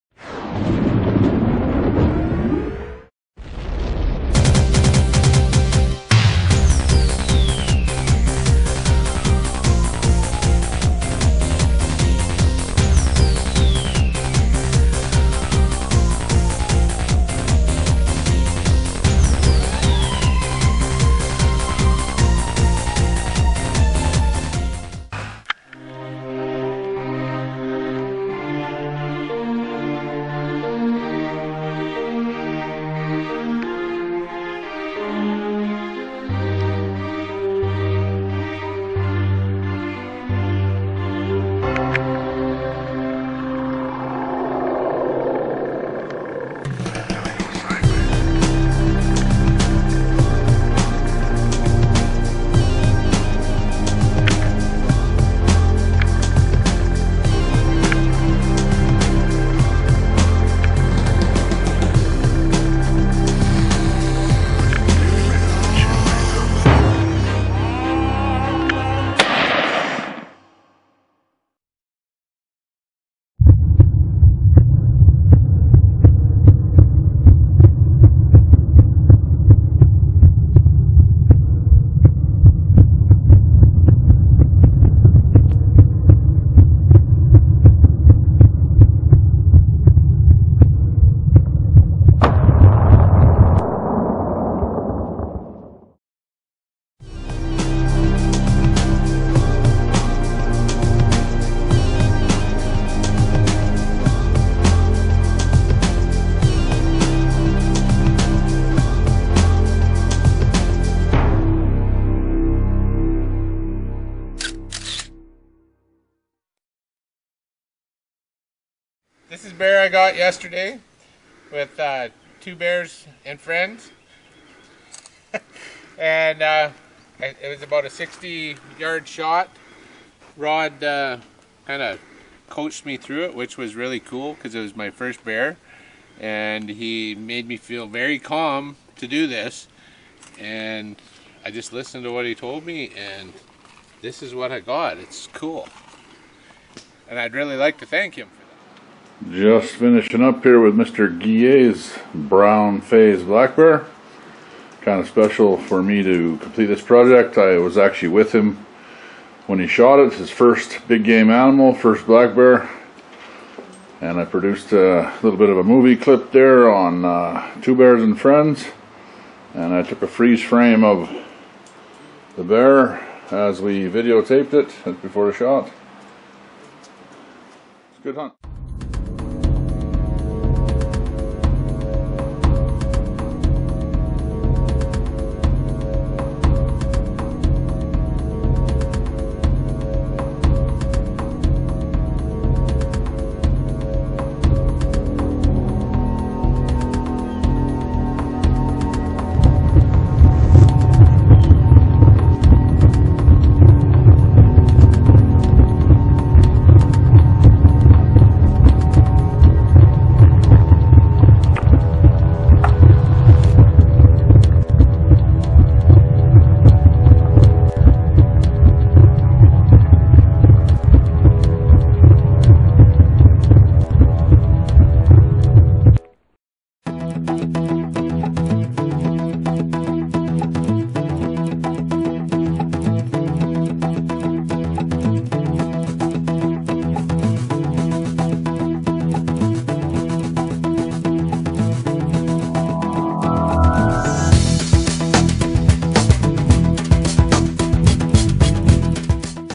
Bear Bone z Black bear taxidermy advertisement
A quick advertisement for Bear Bone-z Taxidermy which contains two black bear kills. One with a rifle and one with a bow. they are tastefull but, graphic. Very good hunting footage. There is also a reminder to convert your existing Hunter Number to the new Fish and Wildlife Identification Number. FWID. Needs to be done starting in April in order to apply for LEH and in short order, to even purchase your hunting license.  No cost for the conversion for existing Hunters Numbers. Can be done on line or at BC Access Center or Vendors of. Website is on this advertisement.